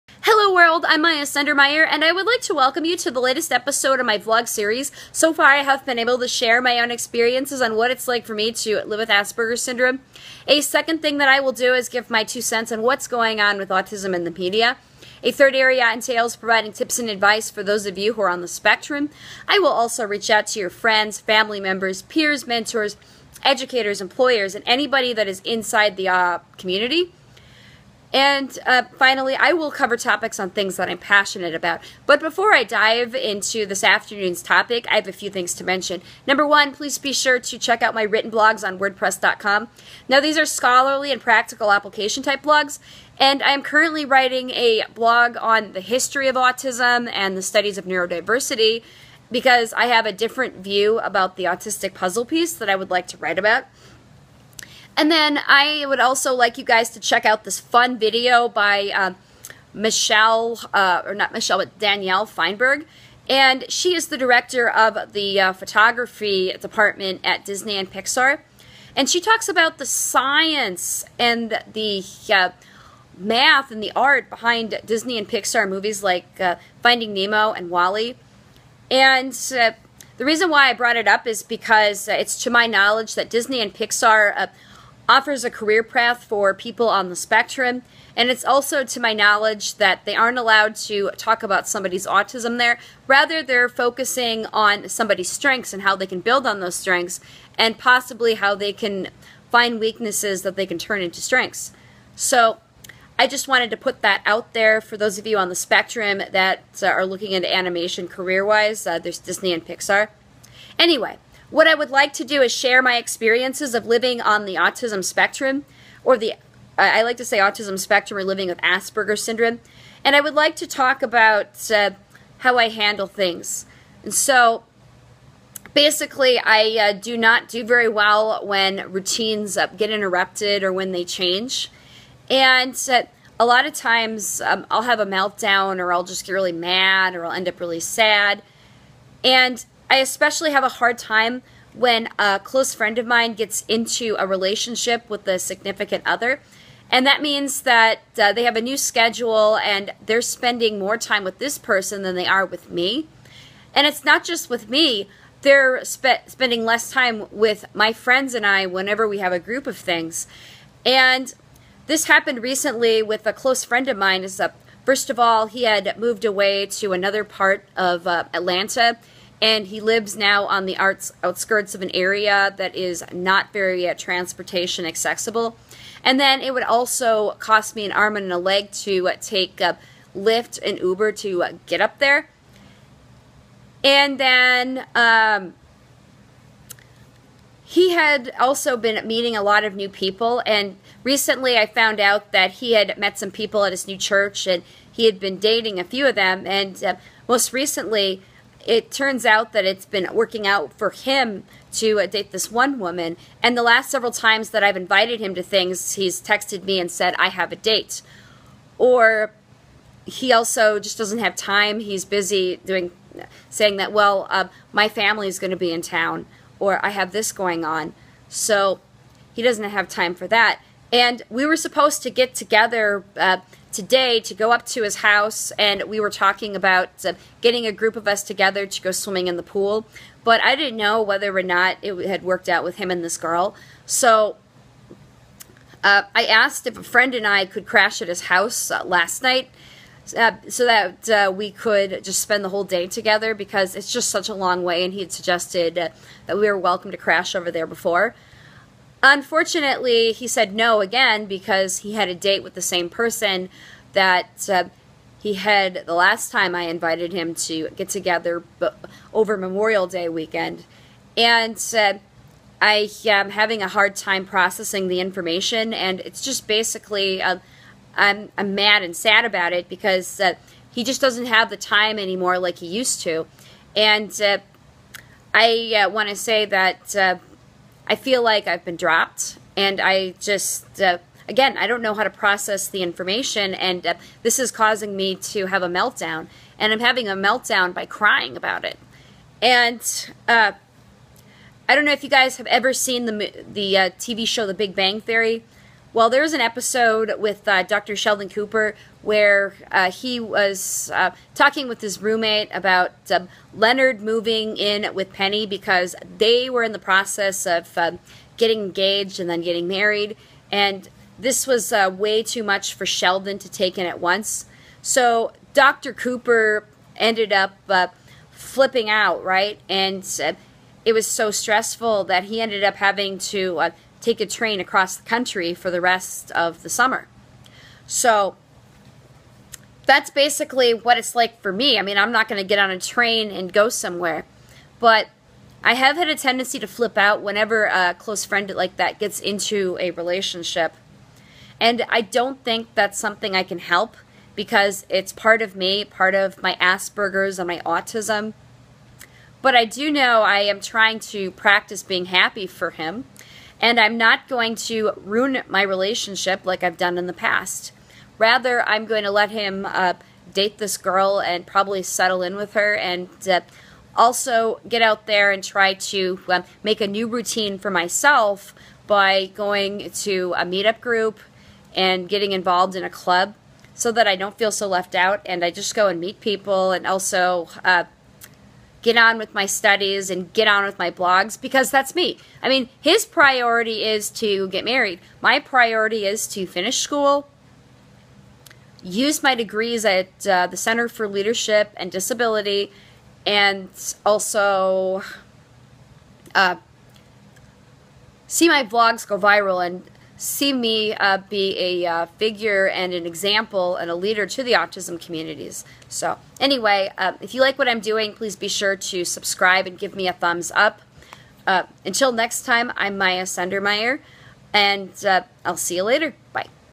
Reacting to my Autistic friend who is dating
One of the most difficult things for me to deal with is a change in routine.  This can be especially stressful when a close friend gets into a relationship with a significant other and suddenly doesn't have time for his or her own routine.  With that being said, I have a friend finally found himself a girlfriend and has had no time for his single friends.  Though I am happy for him, a part of me was mad and sad while recording this as it's difficult to cope.